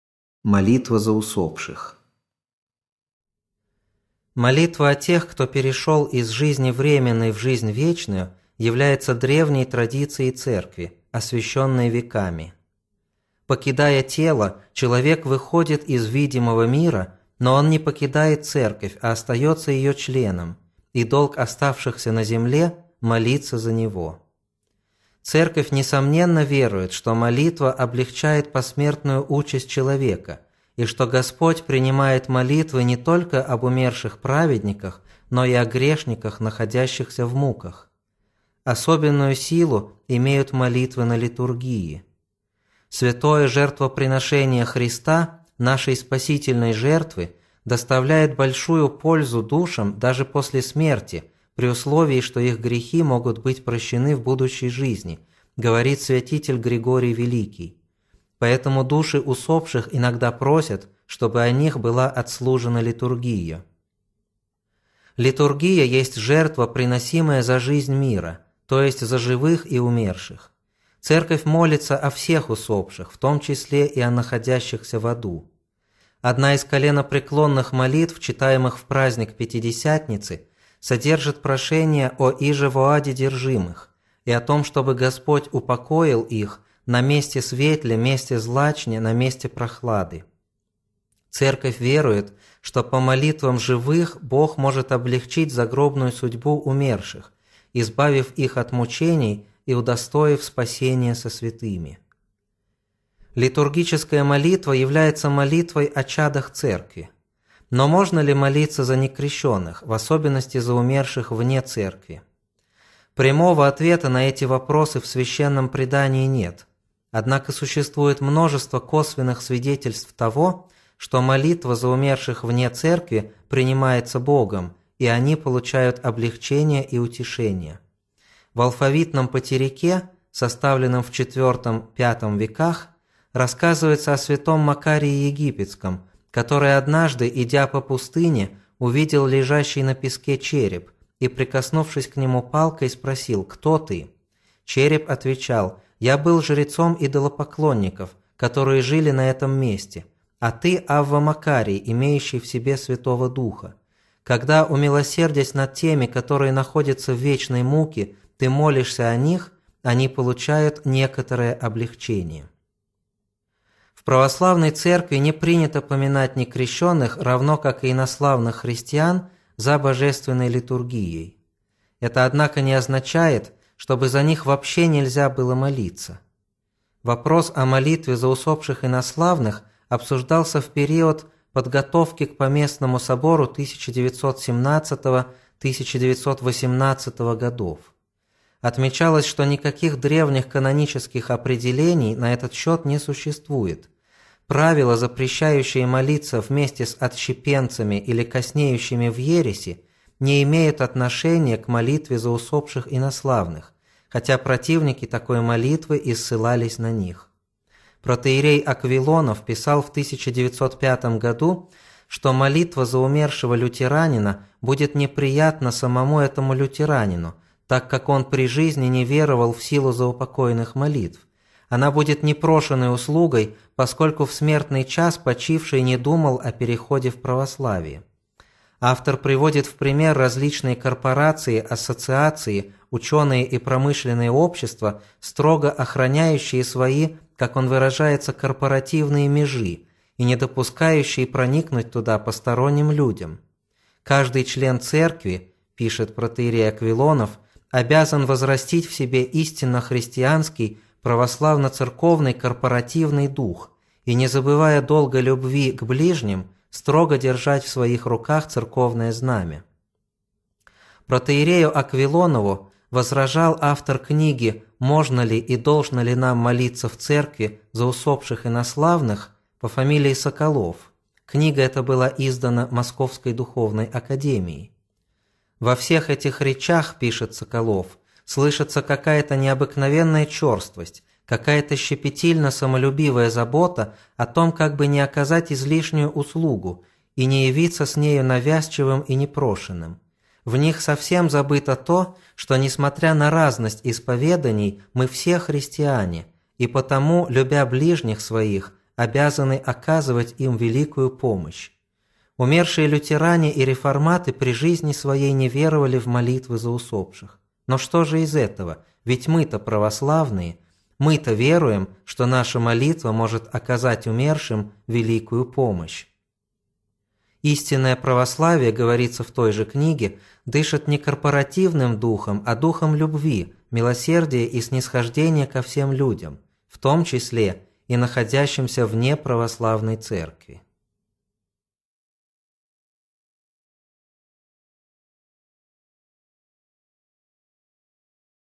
МОЛИТВА ЗА УСОПШИХ ДРЕВНЯЯ ТРАДИЦИЯ ЦЕРКВИ
В этом видео особенности православного вероисповедания, учение о молитве за усопших.
Митр. Илариону Алфееву удается простым и доступным языком объяснить сложные богословские понятия и термины, тем самым делая православие ближе людям, желающим постичь его суть.